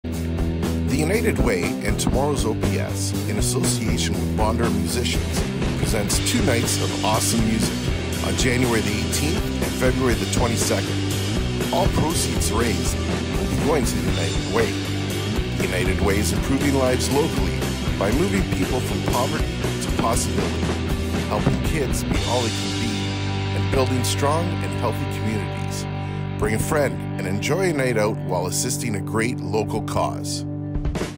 United Way January Jam and United We Jam! Promo
Tomorrow's OPS (Ontario Public Services) along with OLG Musicians present a night of music in support of The United Way in Sault Ste. Maire, ON.